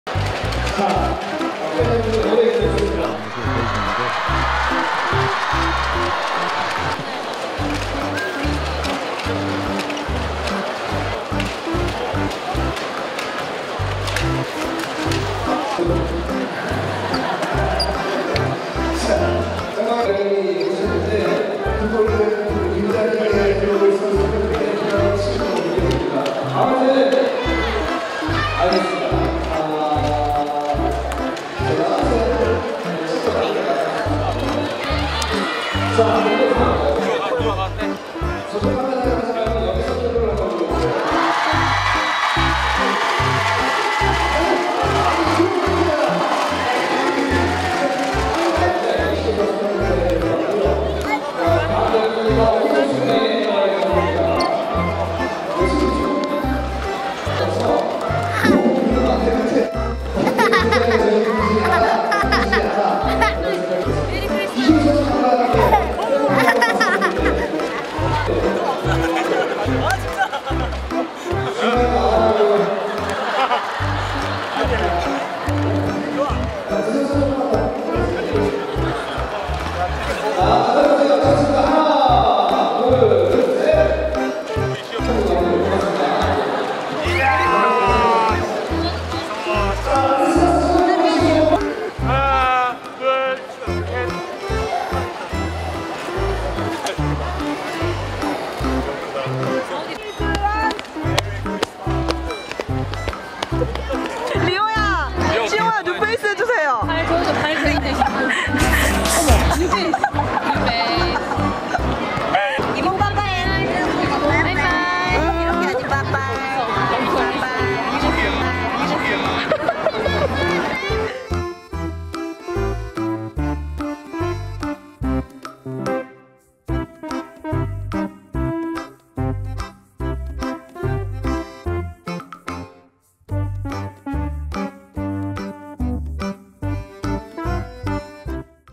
[코다리] 현대캐피탈의 사랑스러운 승리 요정들♥
현대캐피탈 아빠들이 힘이 안 날 수가 없다!
귀염뽀짝 러블리한 승리 요정들!